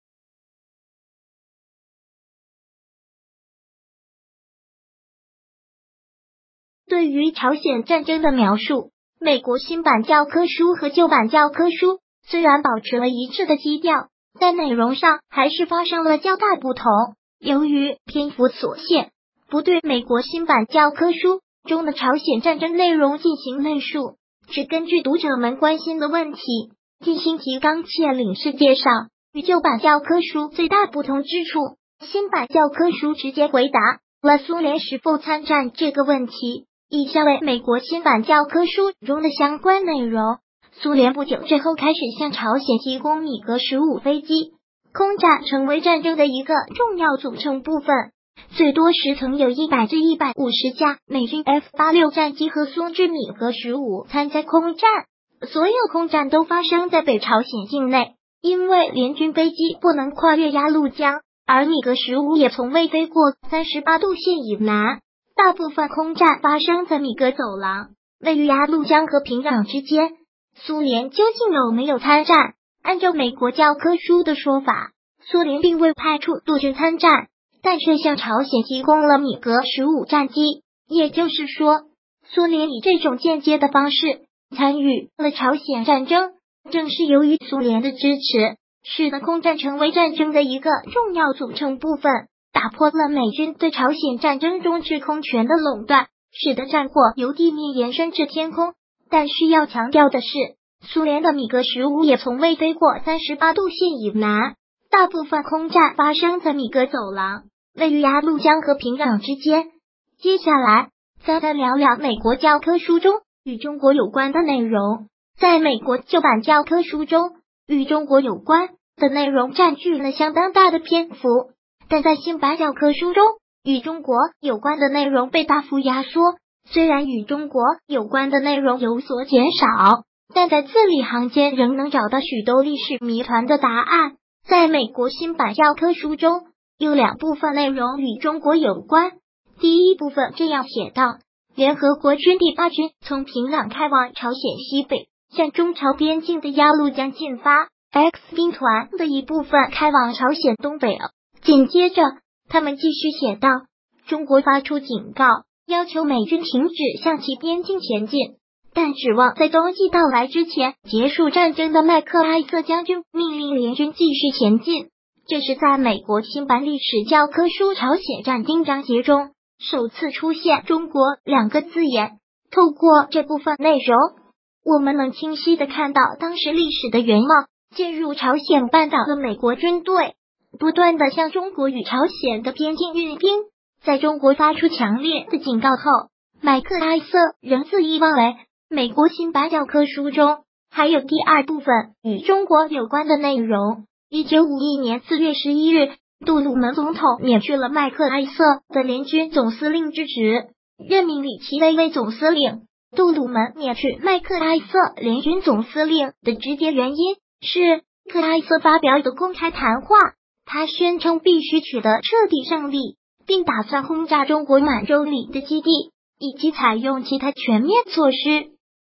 美国新版教科书中的朝鲜战争的一些细节修改
对于朝鲜战争的描述，美国新版教科书和旧版教科书虽然保持了一致的基调，但内容上还是发生了较大不同。由于篇幅所限，不对美国新版教科书中的朝鲜战争内容进行累述，只根据读者们关心的问题，进行提纲挈领式介绍。与旧版教科书最大不同之处，新版教科书直接回答了苏联是否参战这个问题。以下为美国新版教科书中的相关内容：“苏联不久之后开始向朝鲜提供米格15飞机，空战成为战争的一个重要组成部份。最多时曾有100至150架美军F86战机和苏制米格15参加空战。所有空战都发生在北朝鲜境内，因为联军飞机不能跨越鸭绿江，而米格15也从未飞过38度线以南。大部分空战发生在米格走廊，位于鸭绿江和平壤之间。”苏联究竟有没有参战？按照美国教科书的说法，苏联并未派出陆军参战，但却向朝鲜提供了米格15战机。也就是说，苏联以这种间接的方式，参与了朝鲜战争，正是由于苏联的支持，使得“空战成为战争的一个重要组成部份”，打破了美军对朝鲜战争中制空权的垄断，使得战火由地面延伸至天空，但需要强调的是，“苏联的米格15也从未飞过38度线以南，大部分空战发生在米格走廊，位于鸭绿江和平壤之间。”接下来，咱再聊聊美国教科书中与中国有关的内容。在美国